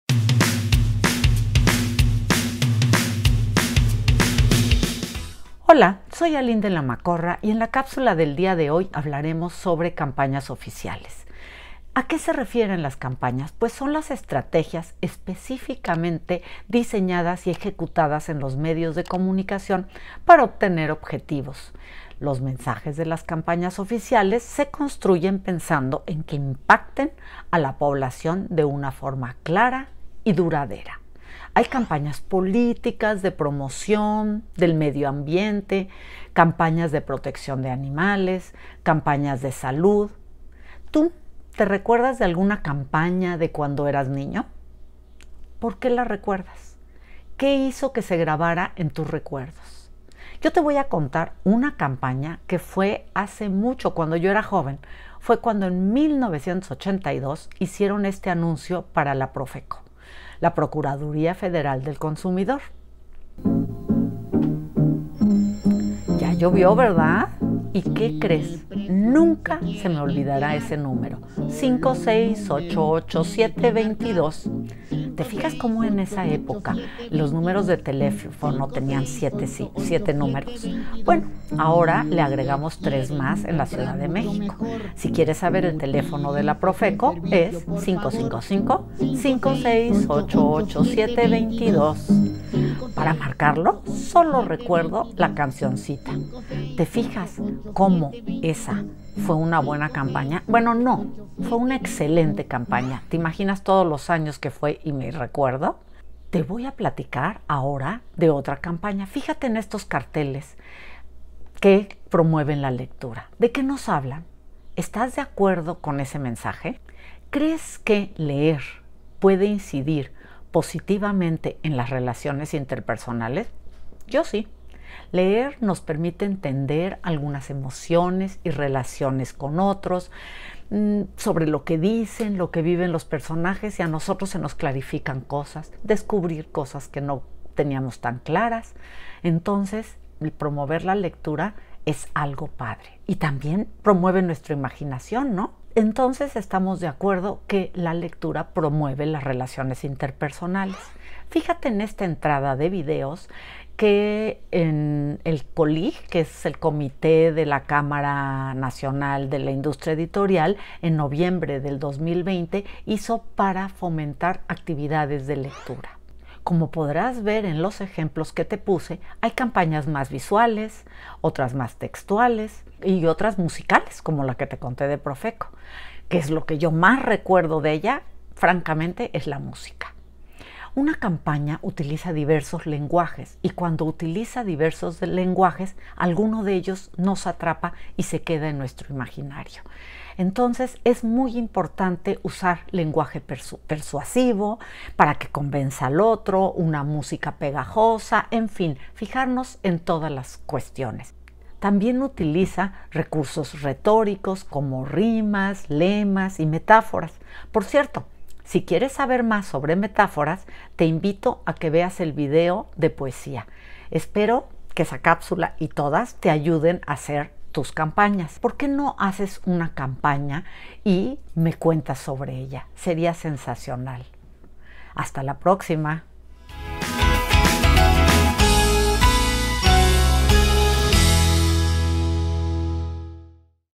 Campañas oficiales
En nuestra cápsula de hoy de Colorines Aprende, hablaremos de las campañas oficiales. ¿Conoces o recuerdas alguna? Aquí te decimos de qué se tratan.